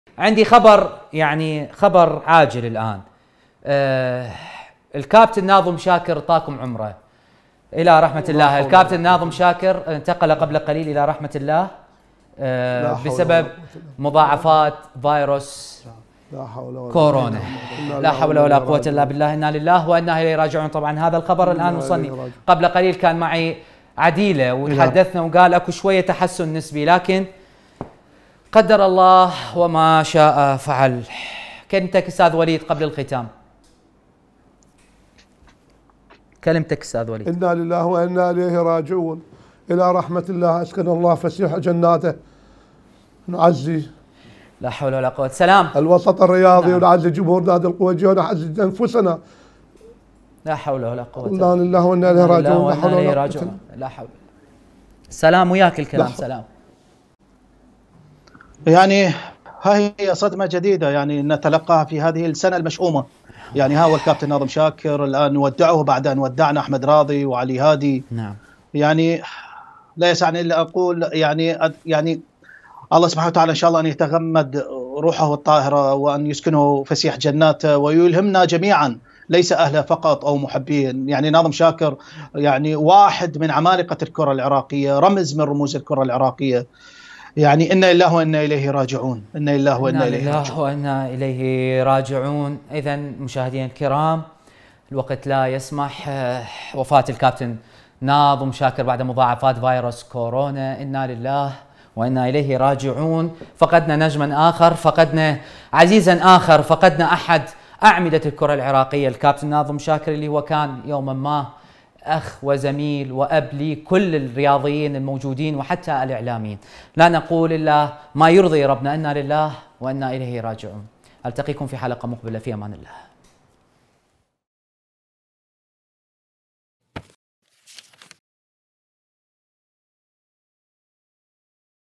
شاهد ردة فعل الضيوف عند تلقيهم خبر وفاة ناظم شاكر على الهواء
ردة فعل الضيوف عند تلقيهم خبر وفاة ناظم_شاكر على الهواء
 لمشاهدة الحلقة كاملة: